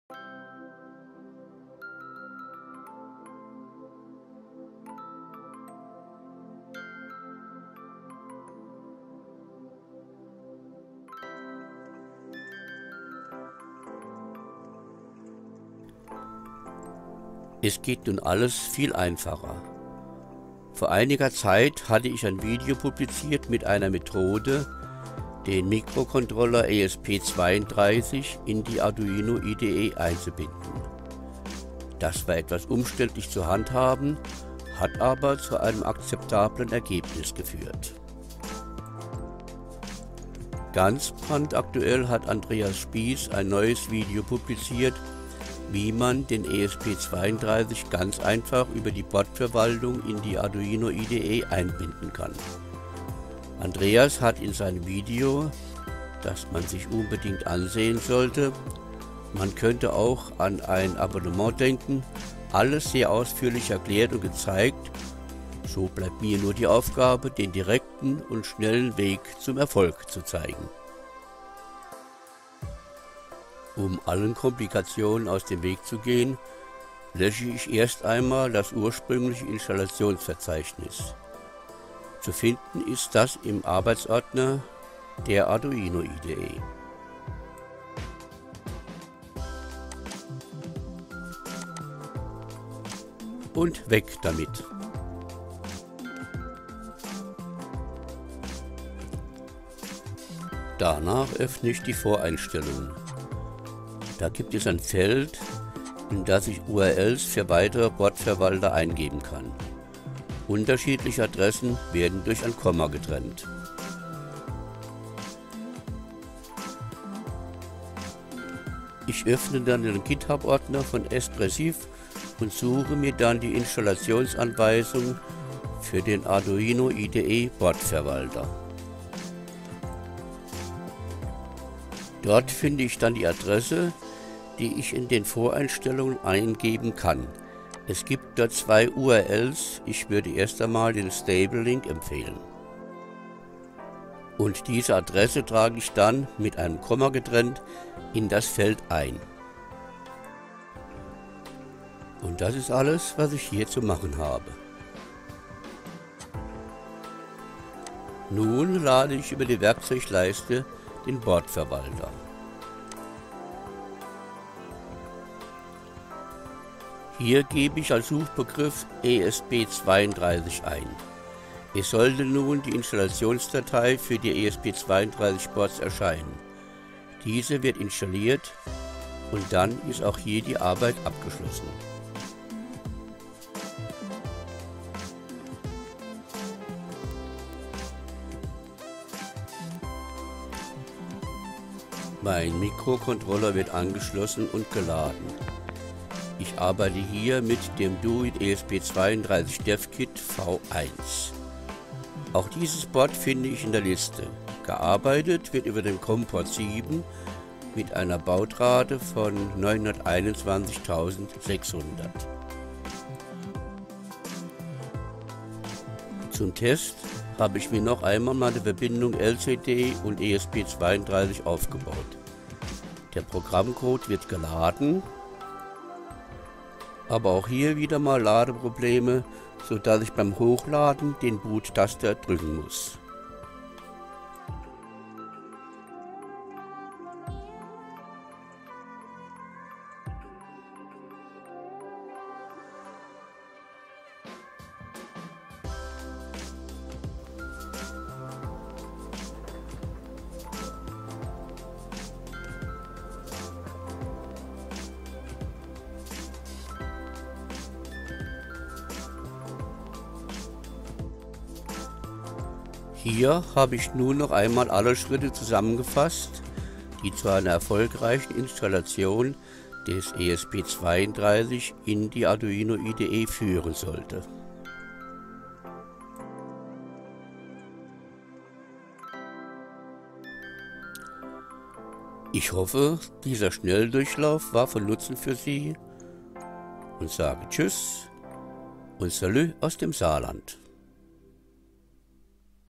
Arduino kurz gefasst - ESP32 und Arduino IDE (Neu)
Es geht nun ganz einfach. Hier stelle ich die neue Mehtode vor, den ESP32 mit Hilfe des Boardverwalters in die Arduino IDE einzubinden.

Die alte Methode über Git-Hub und weitere Tools war doch für einige Benutzer mit Schwierigkeiten  verbunden. Nun geht es ganz einfach und auch schnell.

Die Idee zu diesem Video hat Andreas Spiess geliefert, der über diese Methode schon ein Video in englischer Sprache publiziert hat. Hier ist der Link dazu